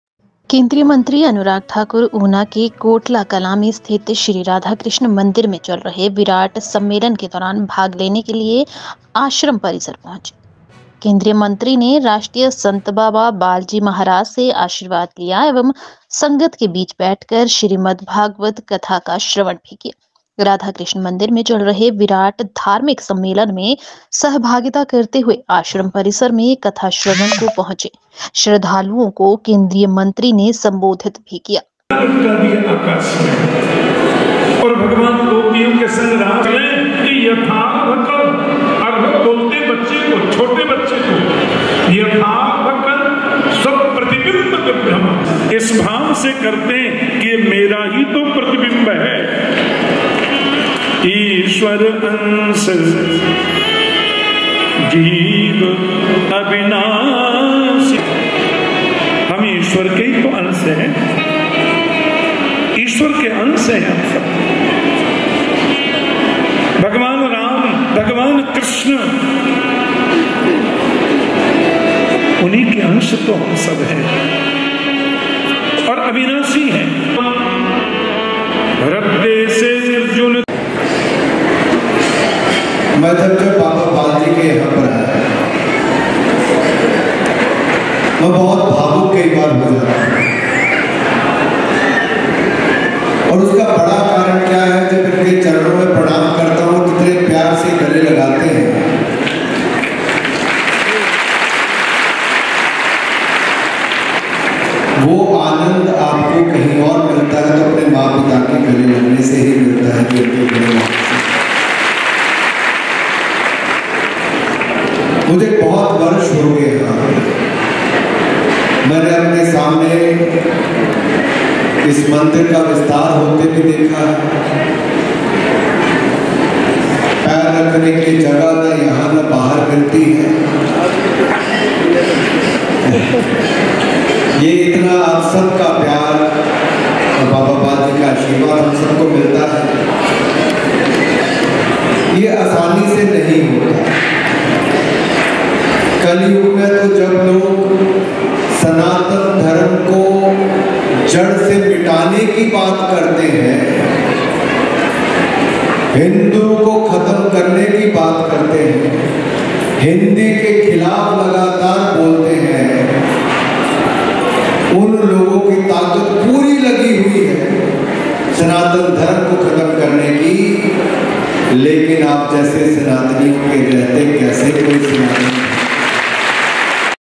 Kotla Kalan/Anurag Thakur/ Baba Bal
Union Minister Anurag Thakur reached the Ashram premises to participate in the ongoing Virat Sammelan at Shri Radha Krishna Temple located in Kotla Kalan, Una. The Union Minister took blessings from National Saint Baba Bal  Maharaj and also listened to Shrimad Bhagwat Katha while sitting among the company.